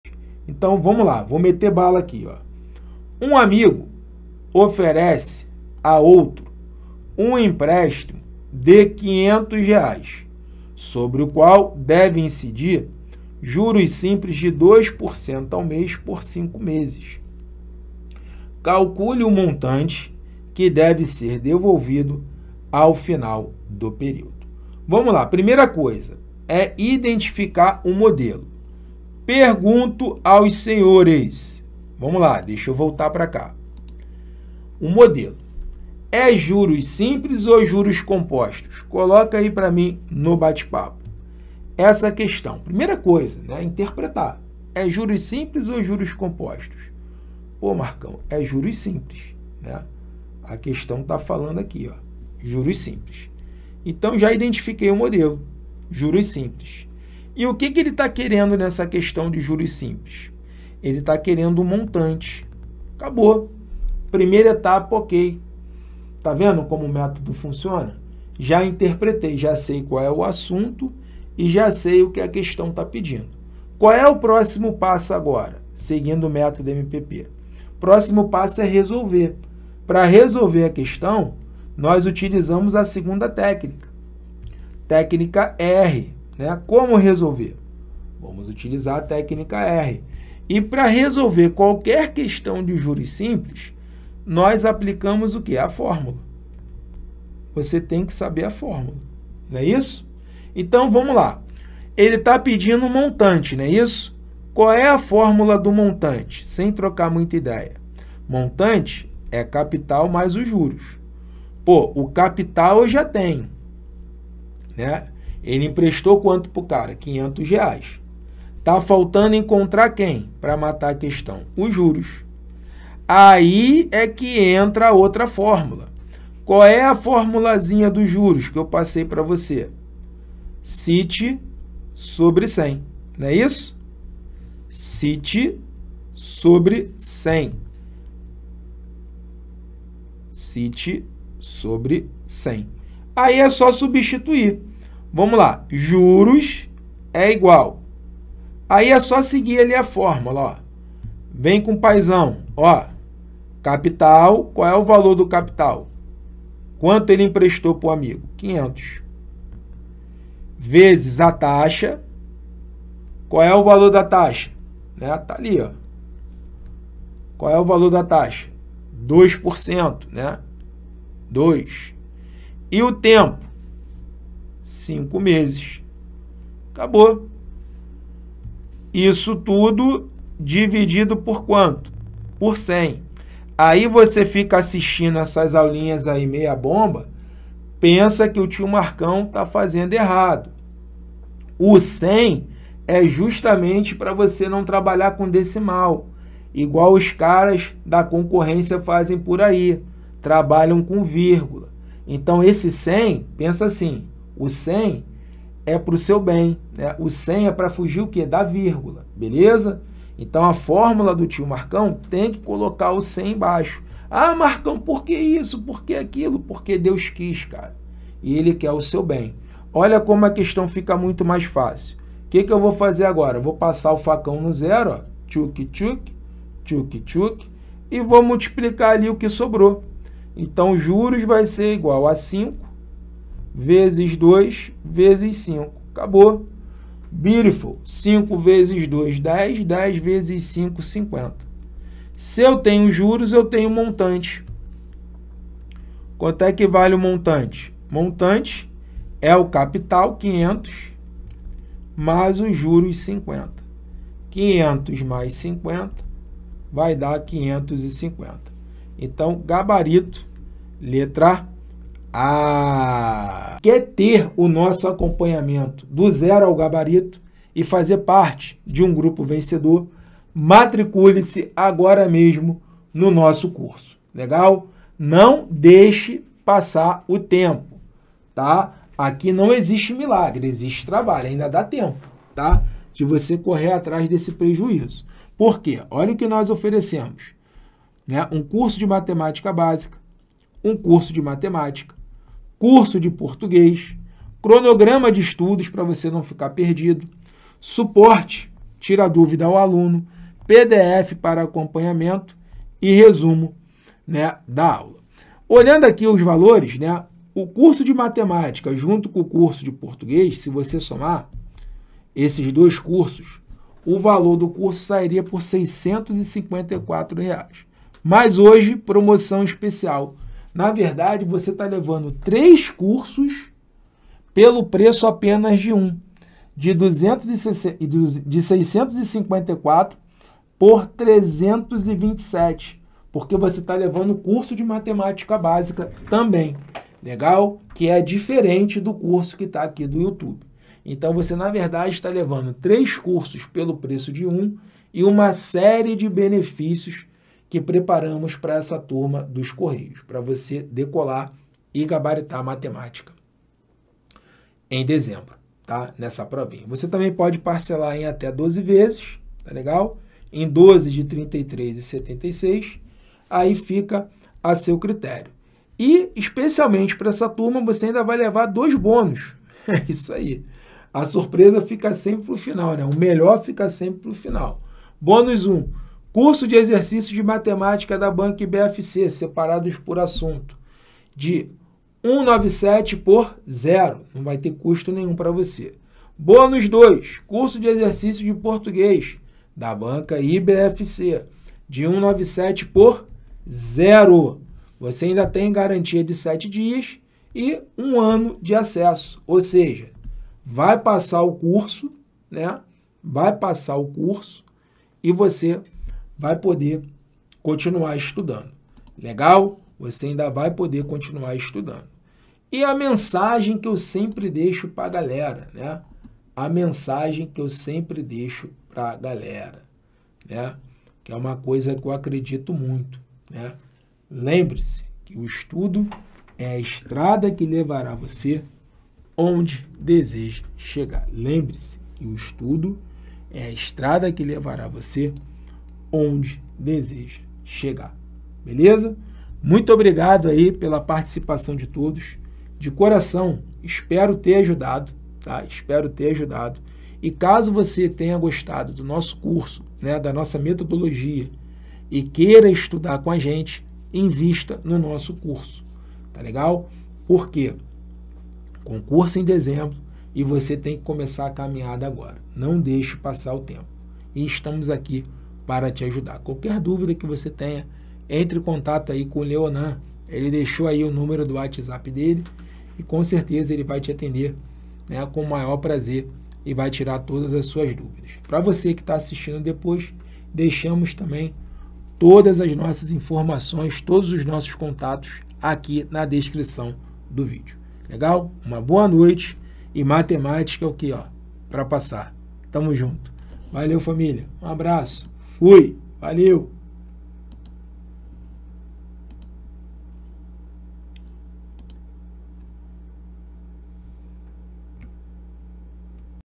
Matemática para os Correios | Não erre mais juros simples
Você precisa assistir esse vídeo onde nosso professor, Marcão, mostra Matemática para Concurso dos Correios 2024 com Método MPP.

Você terá um passo a passo para aprender a matéria mais temida por todos e acertar mais de 80% ou mais nas questões de Matemática para o concurso dos Correios 2024.

Agora basta dar o play no vídeo e aprender Matemática para Concurso dos Correios 2024 com Método MPP